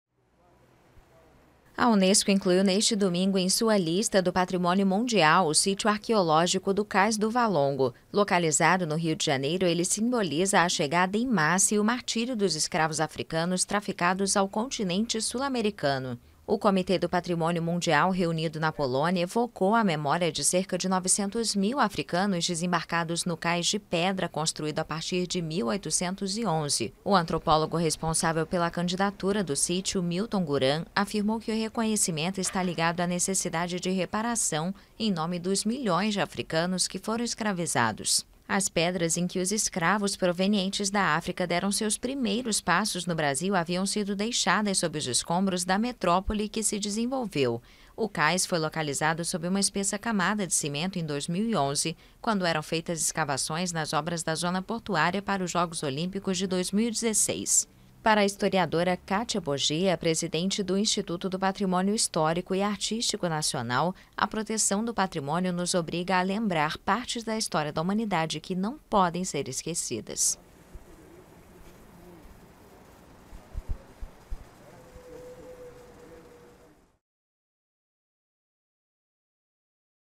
Cais do Valongo é declarado Patrimônio da Humanidade
O sítio arqueológico do Cais do Valongo, no Rio de Janeiro, foi declarado Patrimônio da Humanidade pela Unesco. Este era o local de chegada de milhares de africanos escravizados.